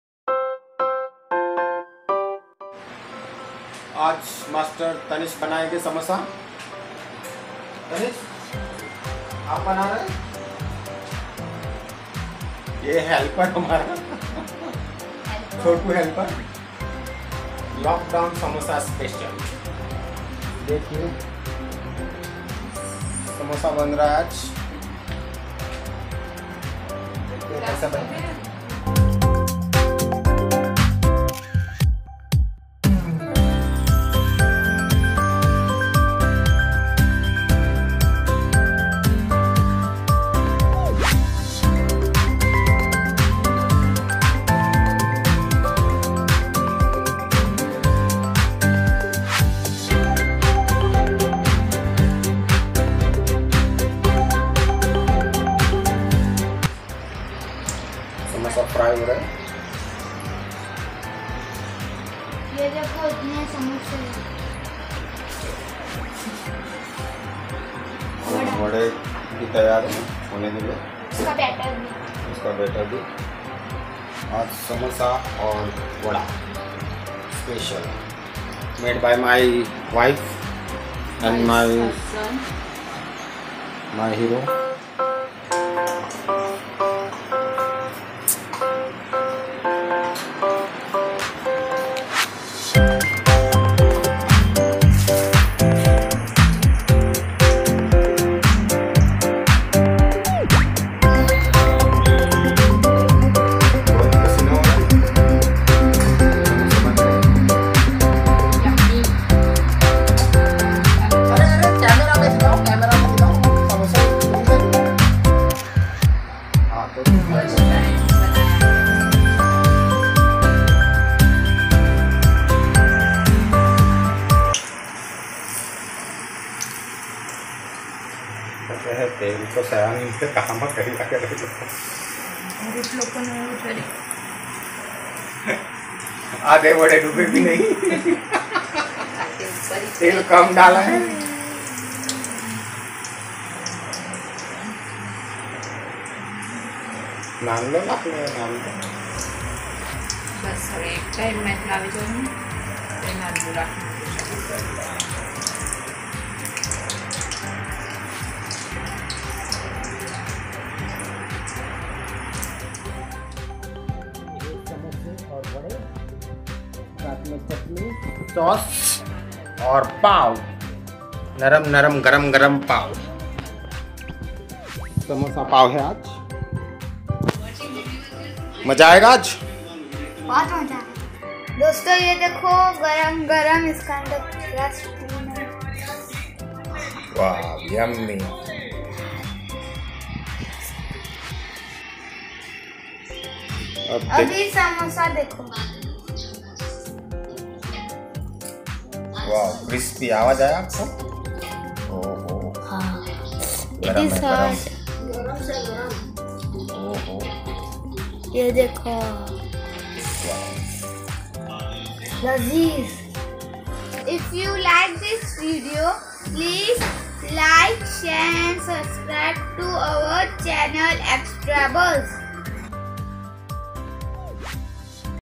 Enjoying making food with my family/ samosa & vada
Extra buzz/ Enjoying making food  with  my family/ samosa & vada